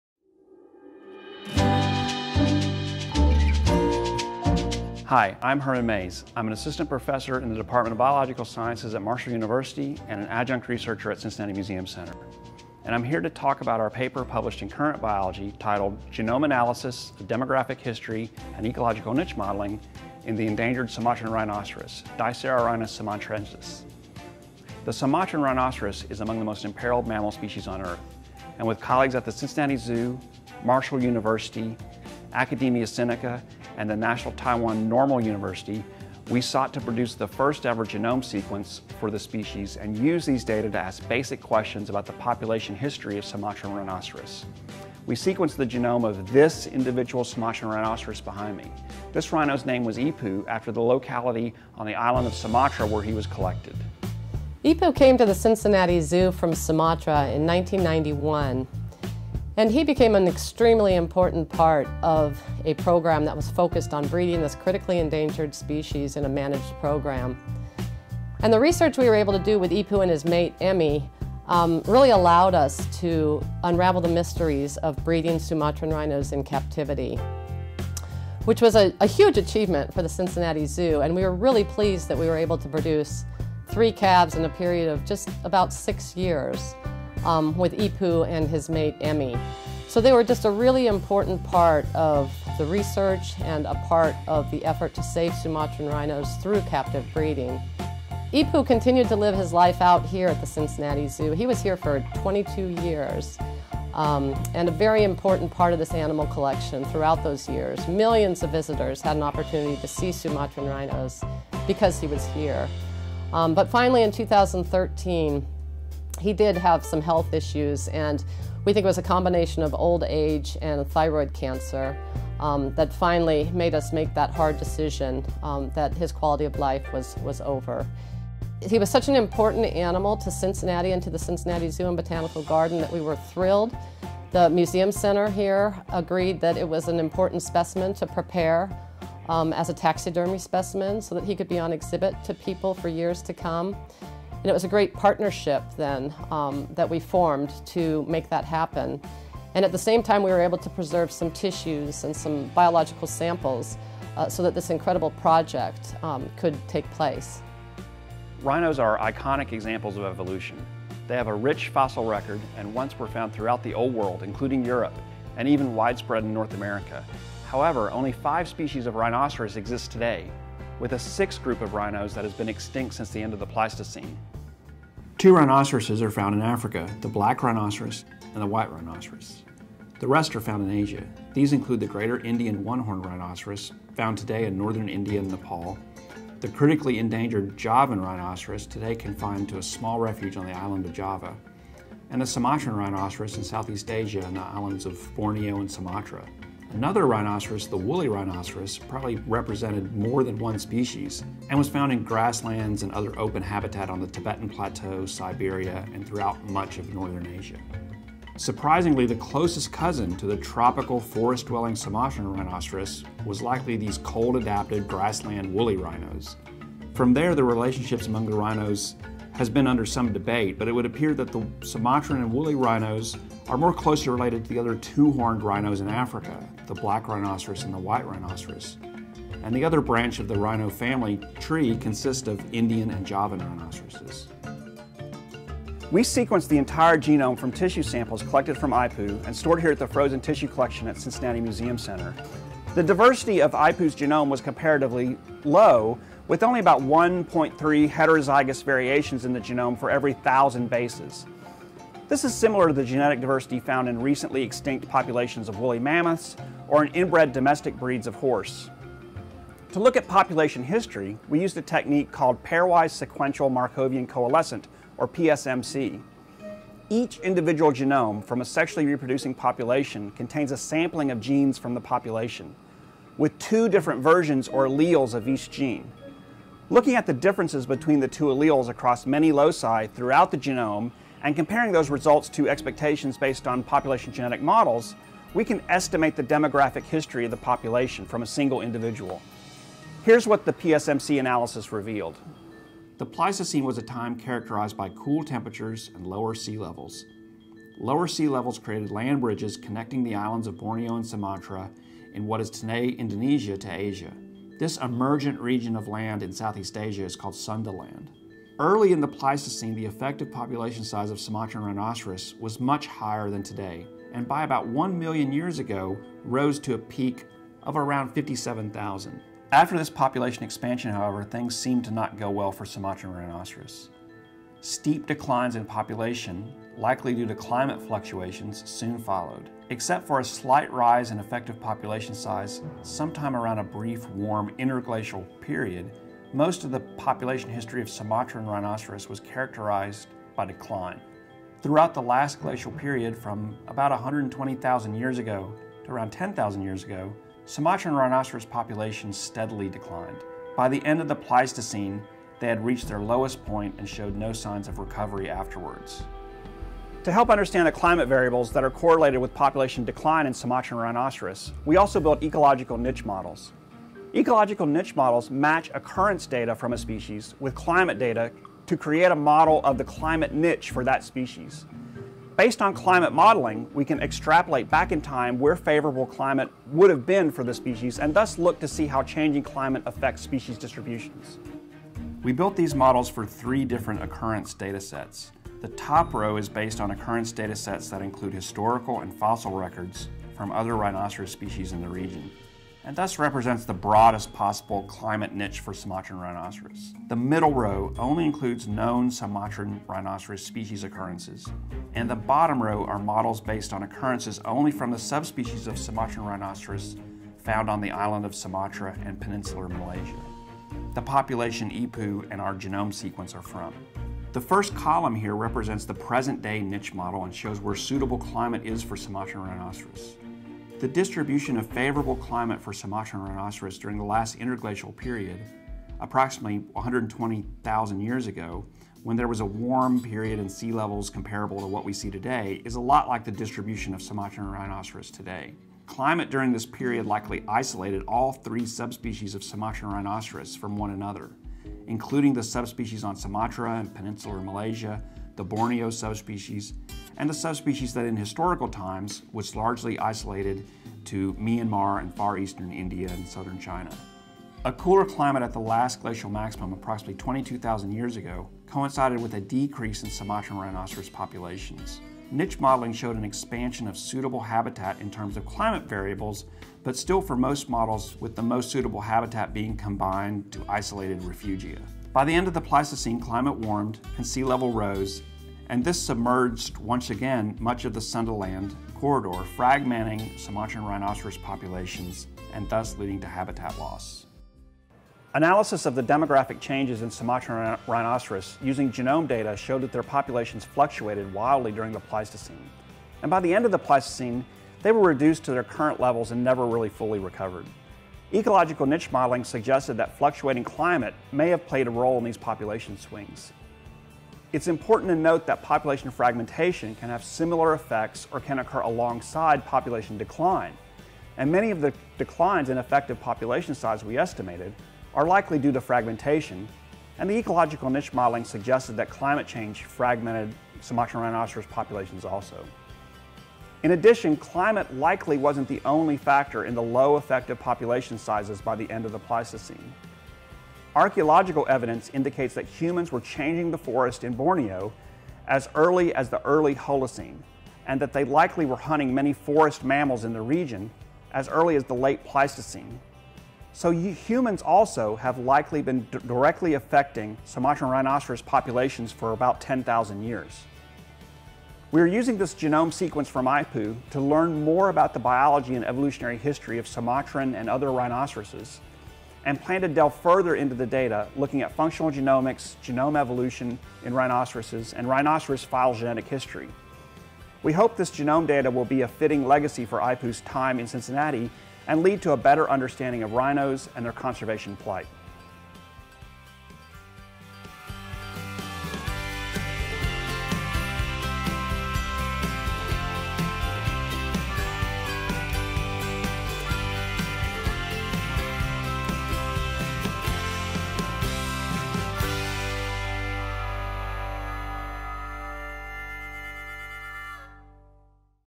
Sumatran Rhinoceros Population History/ Curr. Biol., Dec. 14, 2017 (Vol. 28, Issue 1)
Mays et al. report the first published genome sequence for the Sumatran rhinoceros. Genome data indicated that the demographic history of the Sumatran rhinoceros during the Pleistocene was characterized by extreme fluctuations in effective population size. By the end of the Pleistocene, Sumatran rhinoceros populations plummeted and never recovered. Niche modeling suggests that climate change played a role in these population swings during the Pleistocene.

H.L. Mays, Jr., C.-M. Hung, P.-J. Shaner, J. Denvir, M. Justice, S.-F. Yang, T.L. Roth, D.A. Oehler, J. Fan, S. Rekulapally, and D.A. Primerano (2017). Genomic Analysis of Demographic History and Ecological Niche Modeling in the Endangered Sumatran Rhinoceros Dicerorhinus sumatrensis. Curr. Biol. 28.